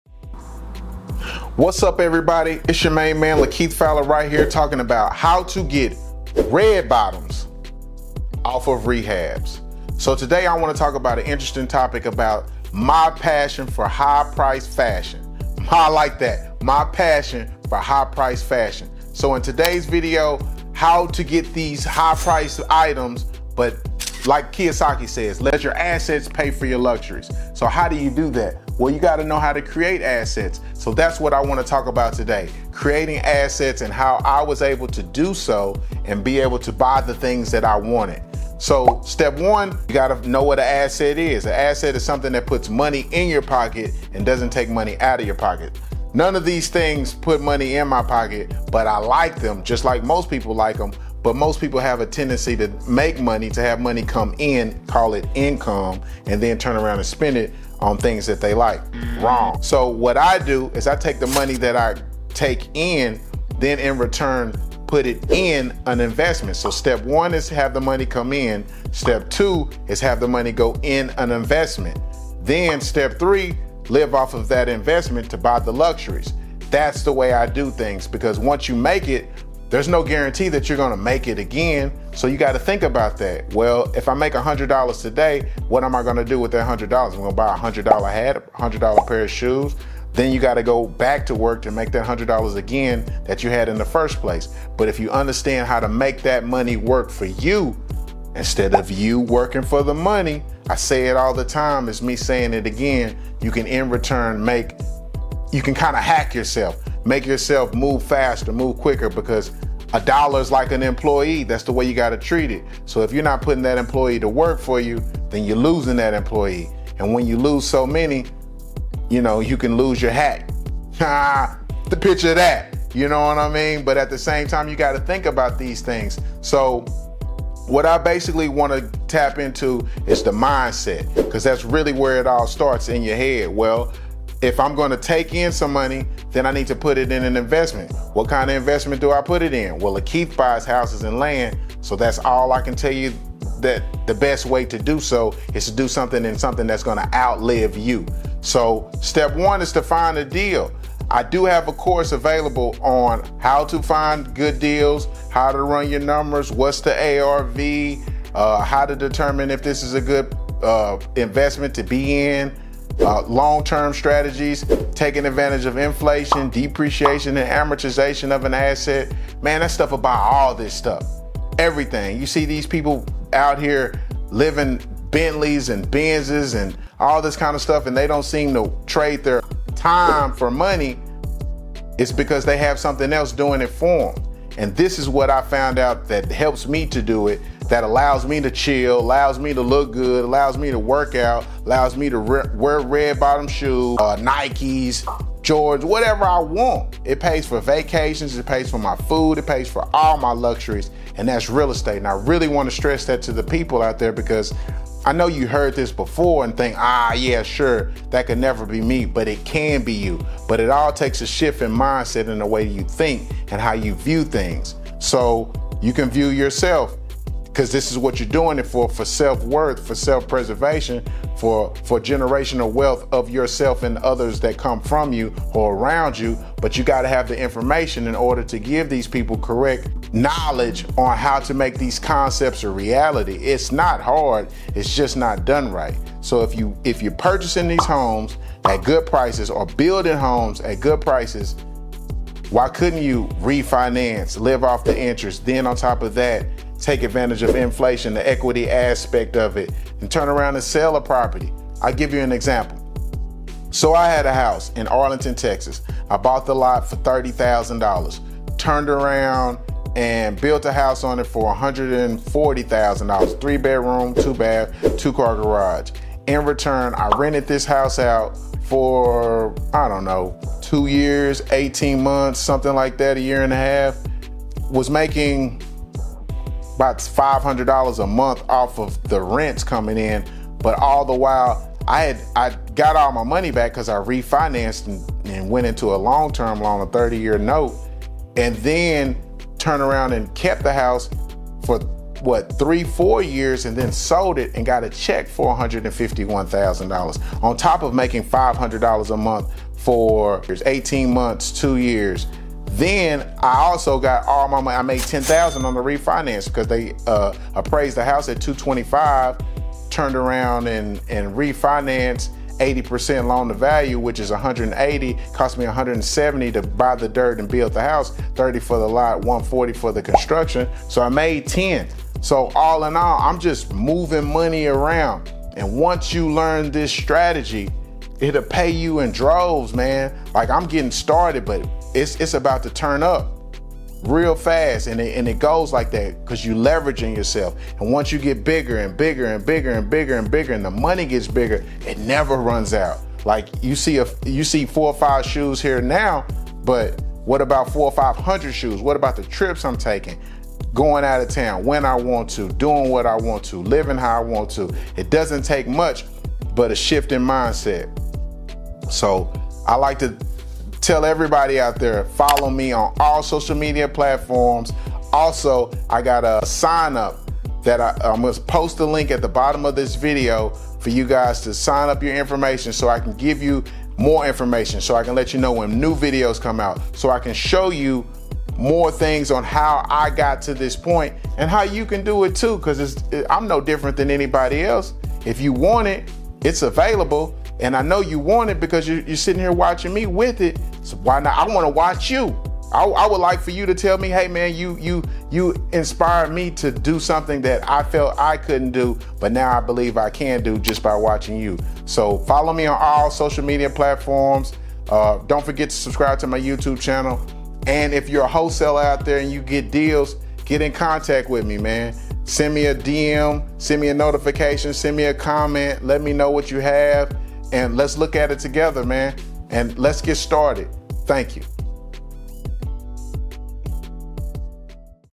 Red Bottoms to Riches|How to Fund Your Luxury Lifestyle with Real Estate
In today's video, we're not just talking about style; we're talking about strategy. It's all about making our assets work for us and living the life we've always dreamed of! 💰

Making money is one thing, but making it work for us is the real game-changer.

For me, the magic lies in the world of real estate . It's not just about houses; it's about creating a legacy.

In this video, we're covering:

✅ Scoring those killer deals
✅ Crunching the numbers
✅ Taking full advantage of inflation, depreciation, and amortization
✅ Building and selling properties for those hefty profits

I began just like anyone else, and I'm here to show you that with the right mindset and knowledge, you can unlock your financial potential too.

Join me on this journey to financial freedom, and remember, you can do it too! Get started with our free step-by-step training at